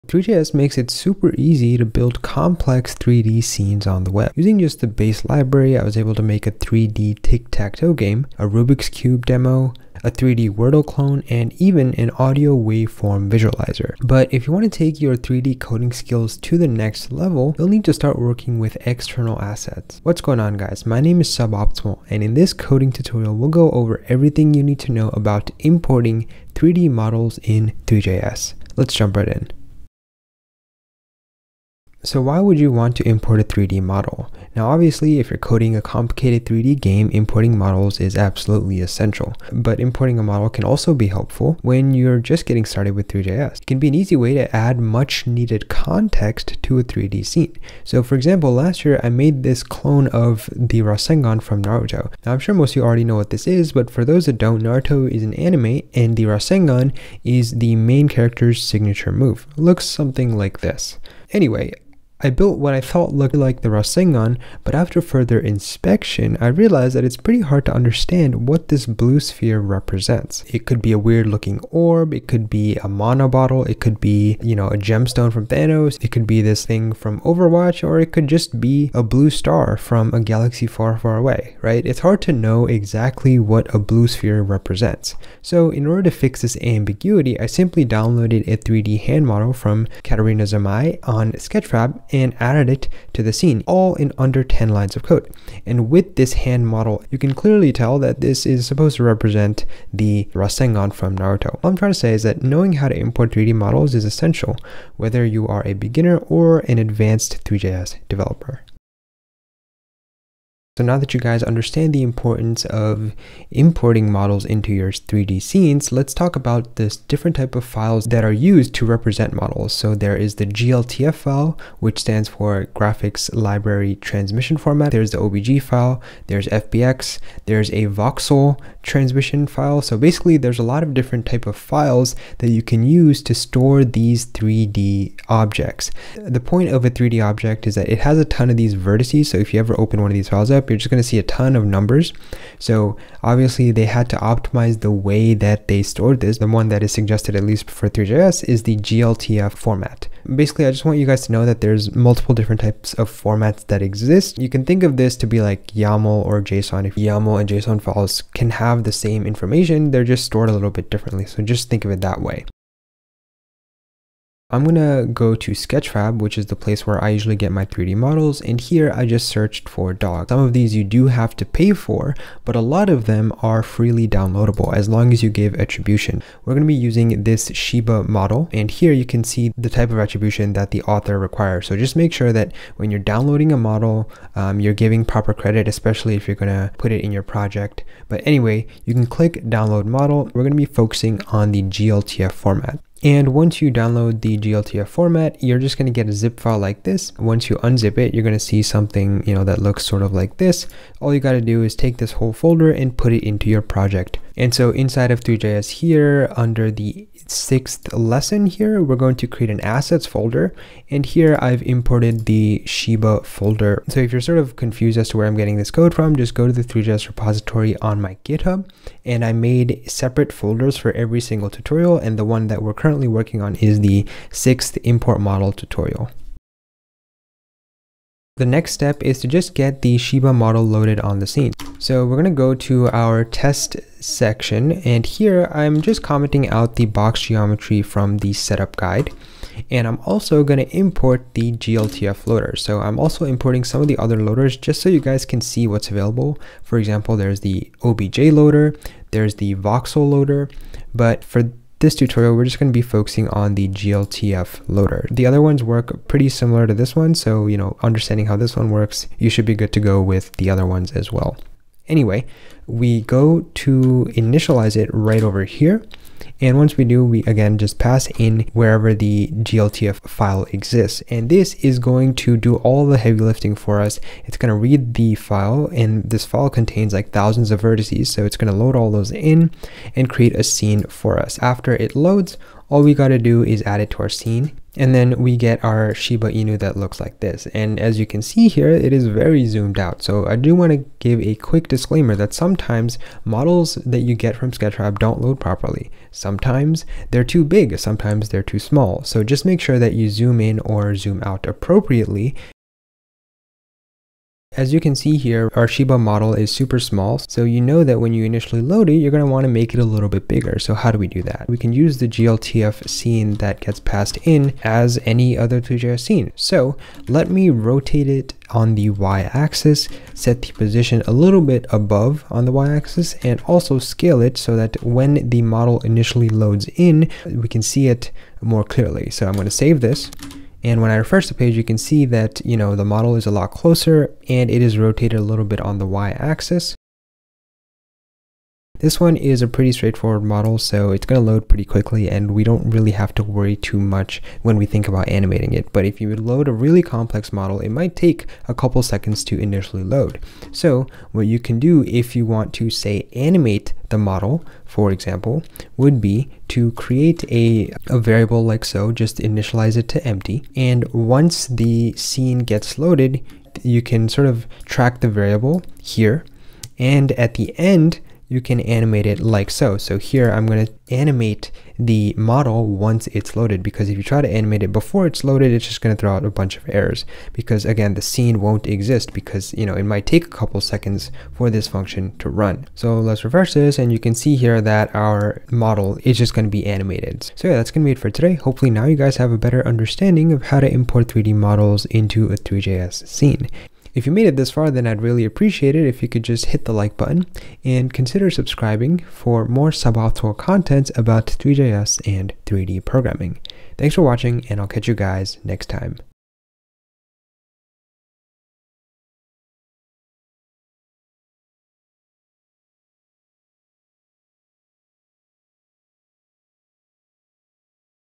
Three.js Importing Models Tutorial | How to Import GLTF Models in Three.js
In this coding tutorial, we go over importing GLTF models in Three JS. We start off by learning when it is useful to import 3D models. Then we go over the different file formats for 3D models like GLTF, OBJ, and FBX. Finally, we find a model on Sketchfab and learn how to import it into our Three.js scene.

== [ Timestamps ] ==
00:00 Intro to Three.js
00:30 Why Use 3D Models?
01:49 File Formats for 3D Models
02:43 Find Model in Sketchfab
03:46 Import GLTF Model
05:03 Transform + Scale Model
05:37 Animate Model